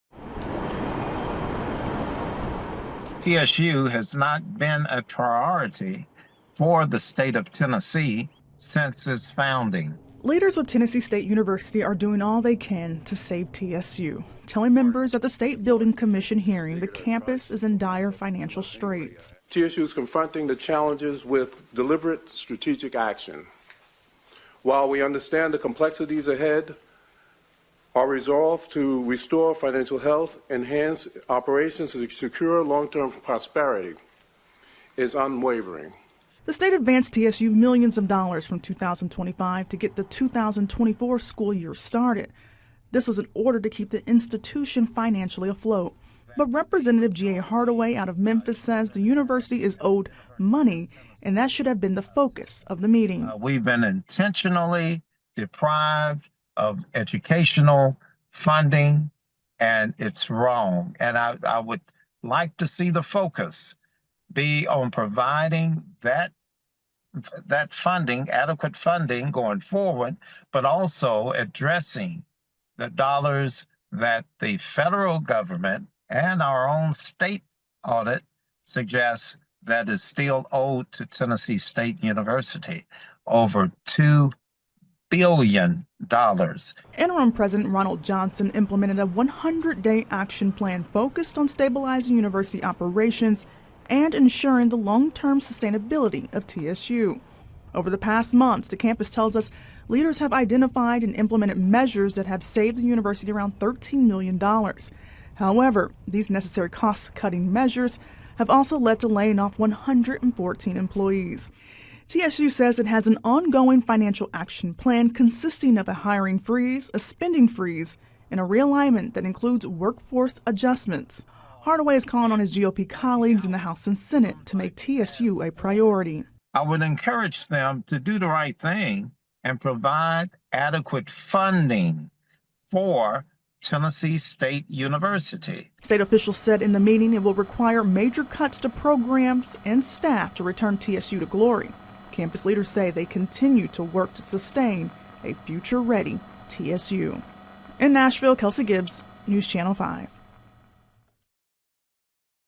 'TSU is owed over $2 billion': Lawmaker calls on state to fully fund Tennessee State University
Leaders from Tennessee State University (TSU) testified before state lawmakers Thursday, warning that the institution is facing a dire financial crisis.